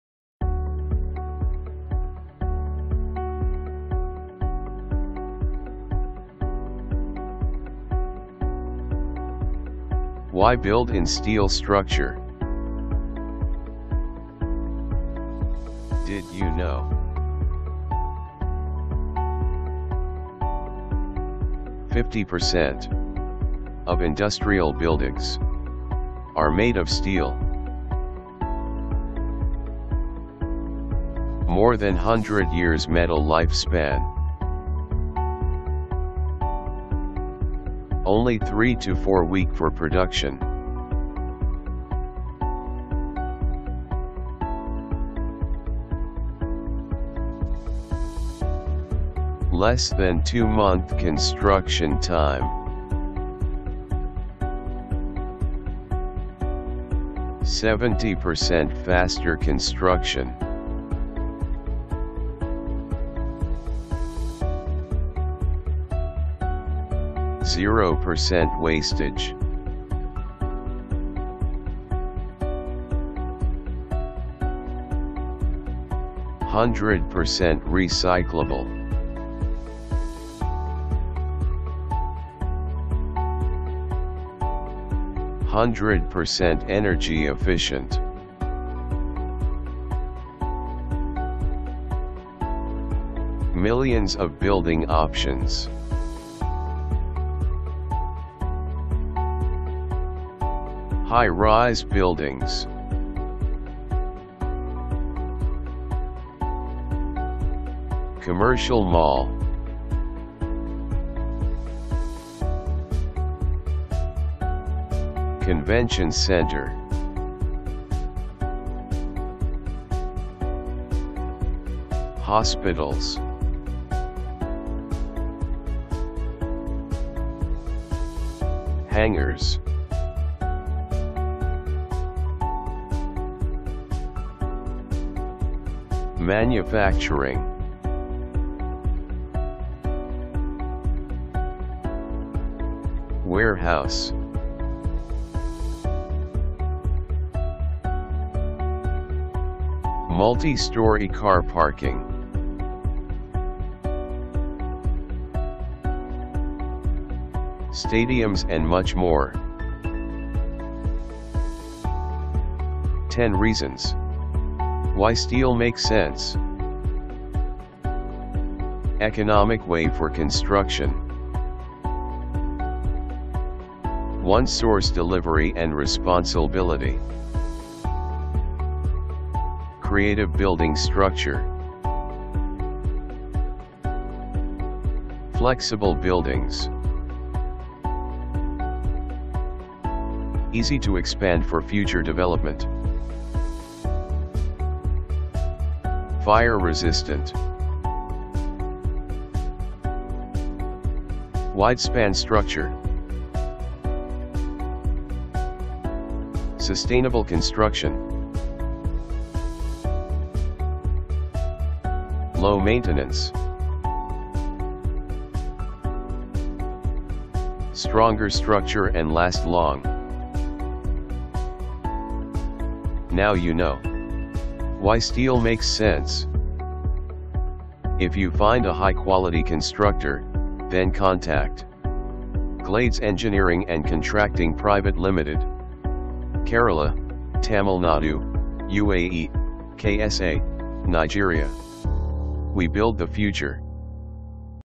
Pre Engineered building, did you know? What is PEB
Glades Engineering and contracting Pvt.ltd.
Is a leading PEB contactor in India.
Show you what is steel structure building and all knowledge about Glades PEB,

watch this full videos to know the basic knowledge about Glades steel structure.
And the future of Pre Engineered Building.

Contact glades engineering and contracting pvt. ltd. :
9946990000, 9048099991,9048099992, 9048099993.